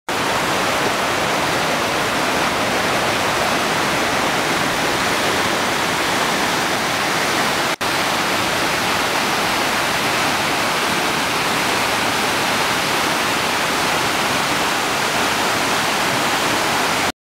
Zealand Falls
Waterfall next to Zealand Hut 05/11/13